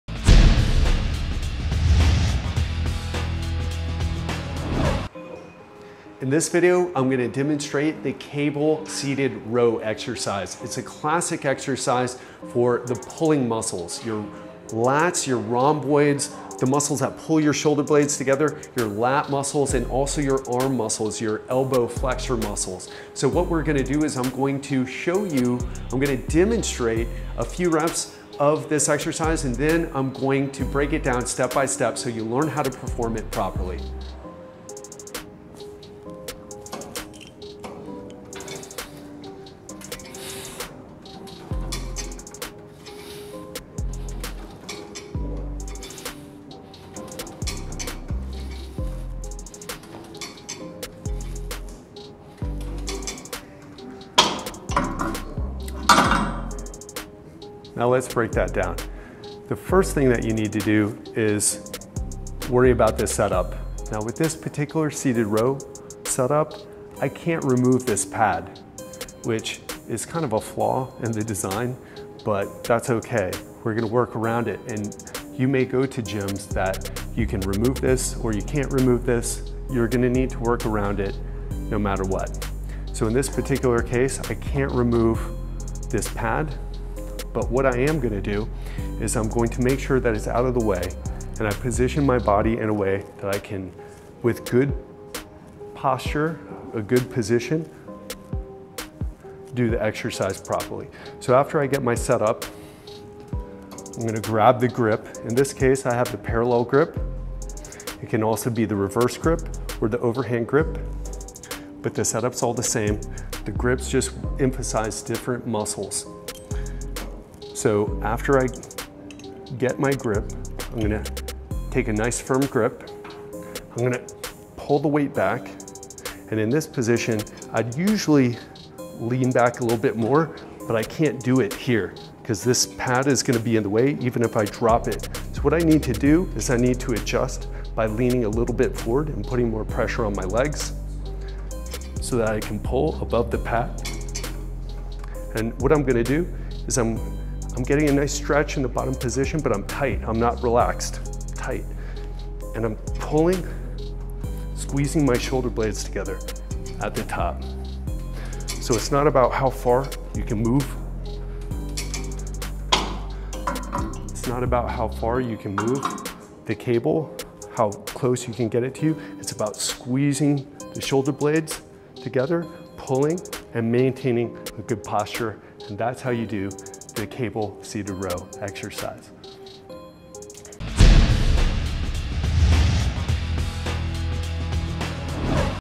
Cable Seated Row
This video will show you everything you need to know about performing the Cable Seated Row exercise. Specifically, you'll learn how to do the Cable Seated Row with a demonstration and explanation from health and fitness expert Ted Ryce.

Instructions:

1. Set Up the Equipment:
   - Adjust the seat and pad (if present) so that it is comfortable and supports your positioning. Note that some setups may not allow for pad removal, so you will need to work around it.
   - Attach the appropriate handle to the cable machine. This can be a parallel grip, reverse grip, or overhand grip, depending on the muscles you want to emphasize.

2. Position Yourself:
   - Sit on the bench with your feet firmly planted on the footrests.
   - Grasp the handle with a firm grip.
   - Sit upright with your back straight and your knees slightly bent.
   - Lean slightly forward if the pad prevents you from leaning back, placing more pressure on your legs to maintain stability.

3. Perform the Cable Seated Row:
   - Start with your arms fully extended, feeling a stretch in your lats while keeping your body tight and engaged.
   - Pull the handle towards your torso, focusing on squeezing your shoulder blades together.
   - Maintain a straight back and avoid leaning excessively backward.
   - Ensure your elbows stay close to your body during the movement.

4. Pause and Return:
   - Pause briefly at the top of the movement, ensuring your shoulder blades are fully squeezed together.
   - Slowly extend your arms back to the starting position, maintaining control and tension in your muscles.

5. Repetitions and Sets:
   - Perform 3-4 sets of 10-15 repetitions.
   - Focus on feeling a contraction in your back muscles and maintaining proper form throughout the exercise.

6. Form and Technique:
   - Keep your core tight and avoid rounding your back.
   - Move in a controlled manner, avoiding using momentum to pull the weight.
   - Focus on the quality of each repetition rather than how far you can move the handle.

7. Weight and Progression:
   - Start with a manageable weight to focus on form and muscle engagement.
   - Gradually increase the weight as you become more comfortable with the movement.
   - Stay within the 10-20 rep range to prevent overloading and ensure proper focus on the back muscles.

8. Usage and Benefits:
   - Use this exercise to strengthen your lats, rhomboids, and other muscles involved in pulling movements.
   - It is especially beneficial for improving posture and upper back strength.
   - Incorporate it into your routine to enhance your performance in other pulling exercises like pull-ups and bent-over rows.

9. Safety Tips:
   - Avoid using too much weight initially to prevent using your biceps instead of your back muscles.
   - Focus on maintaining proper form to avoid lower back strain.
   - Listen to your body and stop if you experience any pain or discomfort.

Give the Cable Seated Row a try, ensuring you set up correctly, maintain proper form, and focus on engaging your back muscles.